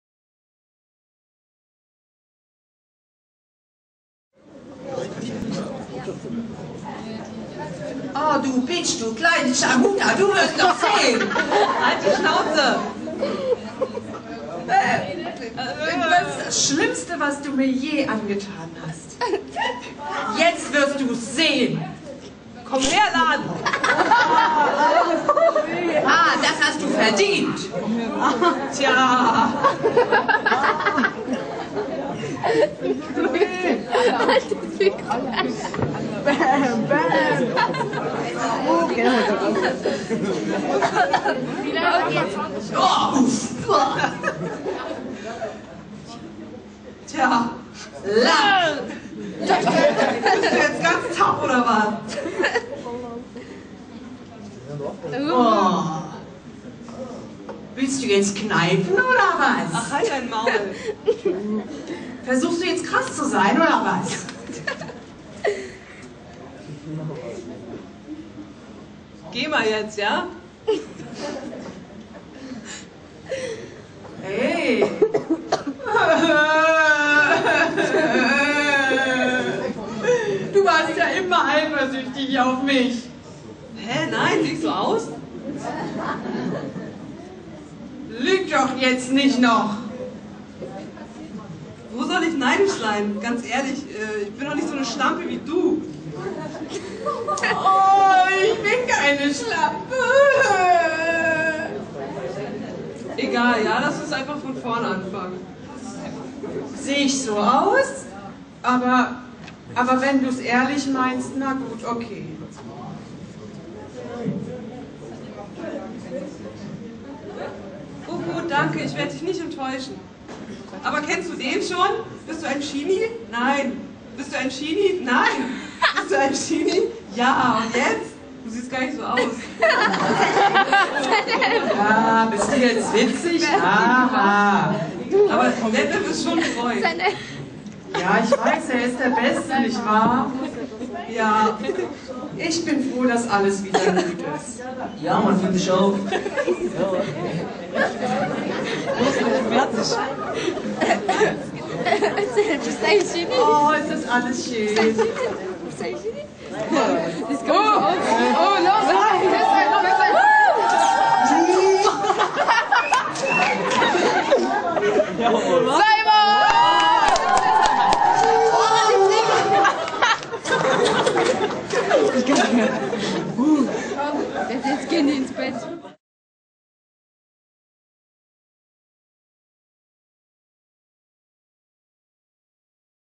Wääääh
Machinima, entstanden auf der play11 - 4. Festival für kreatives Computerspielen. Produziert von Melissa & Nida aus der "Schule an der Jungfernheide"